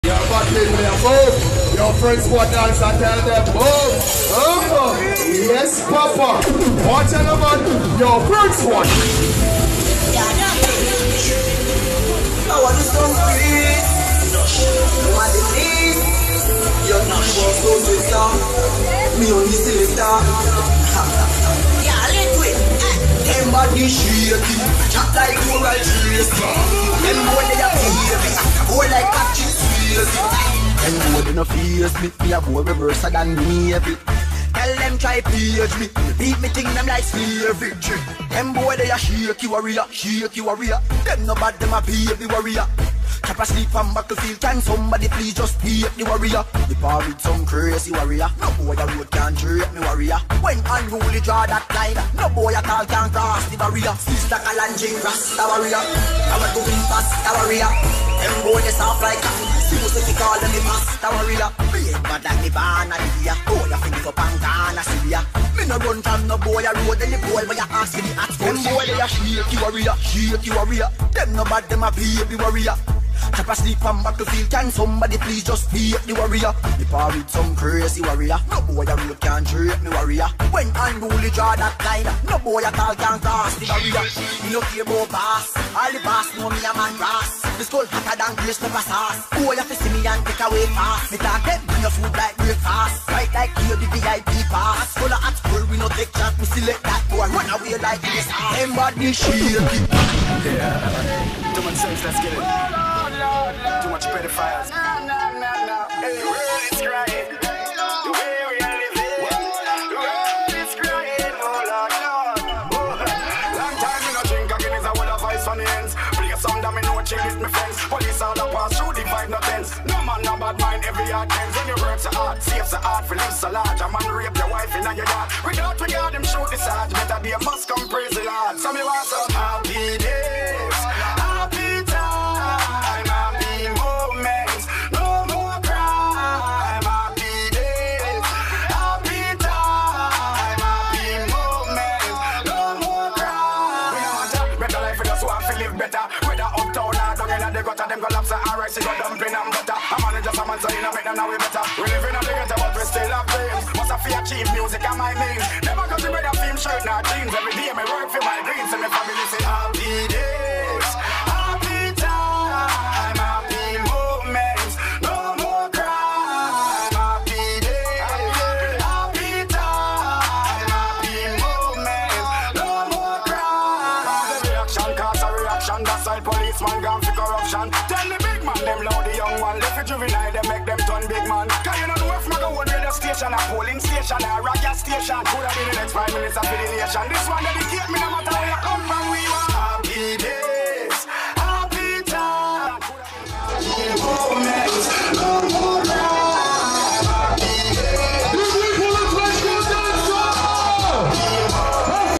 FSD - French Squad Danca - Showcase - DanceSoldiah 10thAnniversary
FSD - DanceHall showcase - Cabaret Sauvage - DanceSoldiah10th Anniversary - Mighty Crown - Paris

CONTACT twitter F.S.D Instagram : @frenchsquaddanca @suns_frenchsquaddanca @hennessytuoshean_frenchsquad